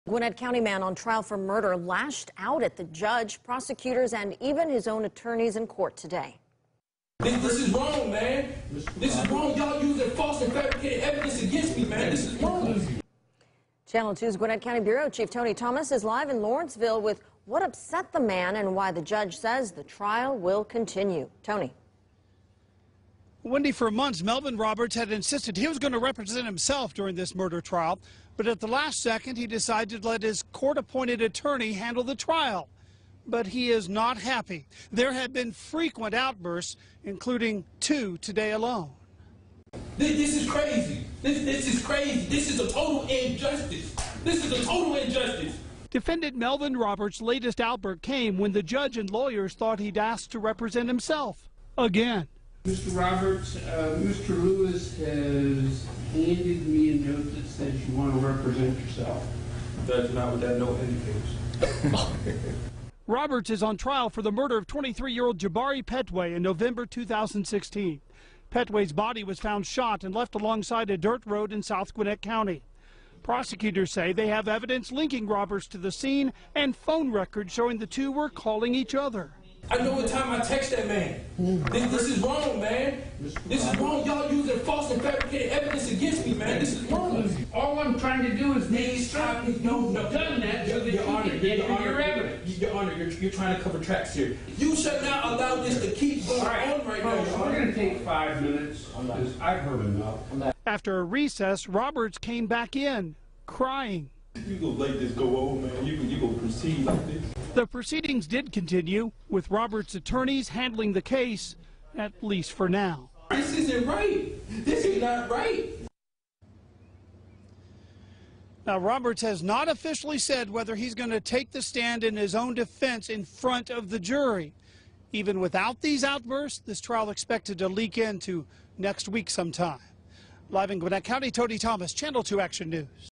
Gwinnett County man on trial for murder lashes out against judge, prosecutors
The man was found guilty on all counts.